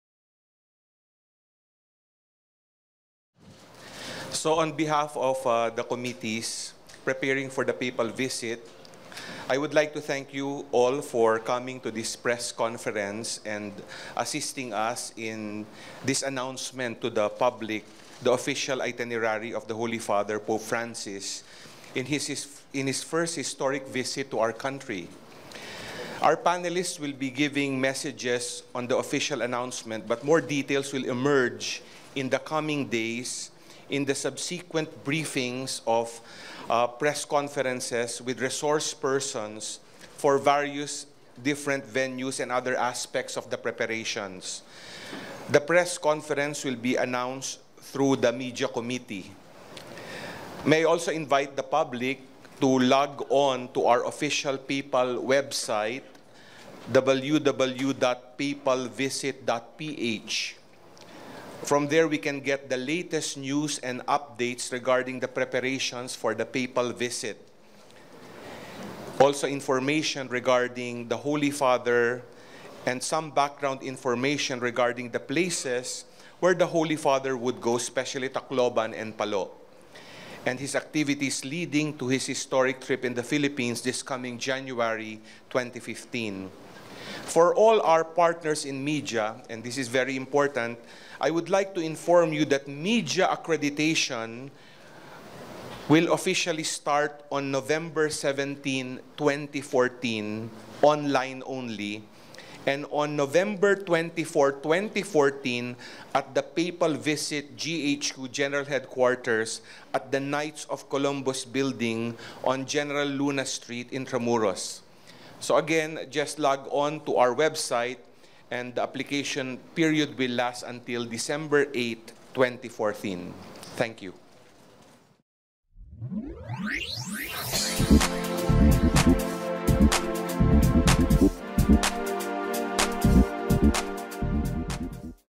Media Accreditation for Papal Visit 2015
At the press conference held on November 14, Pasig Bishop Mylo Hubert Vergara, head of the CBCP Commission on Mass Media,  announced the media coverage of the Pope's Visit and  media accreditation that will start on November 17, 2014;  he also announced that the General Headquarters of the Media Committee for the Pope's Visit will be at the 3rd Floor of the Knights of Columbus Building at Gen. Luna Cor Sta. Potentiana Sts., Intramuros Manila.